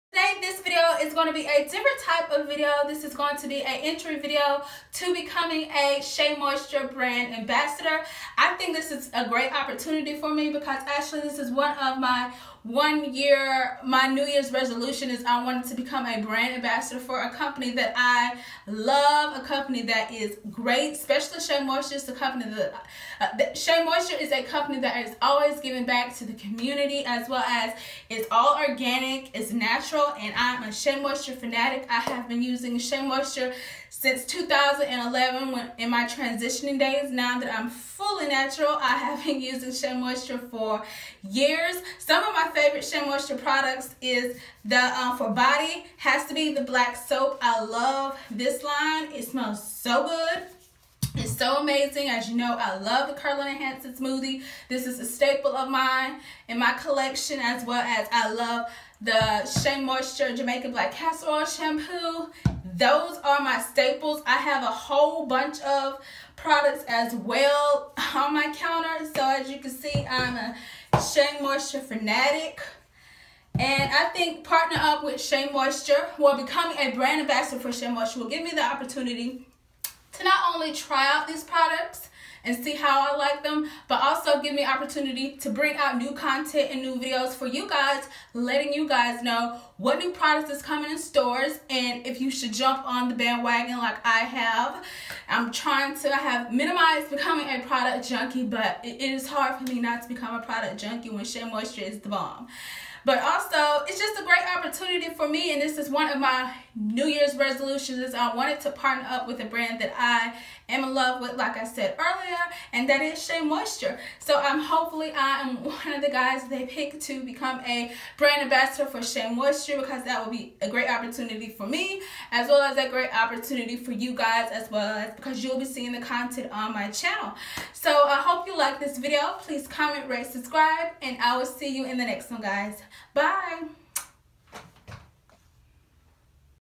Shea Moisture Brand Ambassador Entry 2017
Hey guys this is my entry video to hopefully becoming a brand ambassador for Shea Moisture I think that this would be a great opportunity for me and you guys because I will be able to bring you ladies new content on my channel .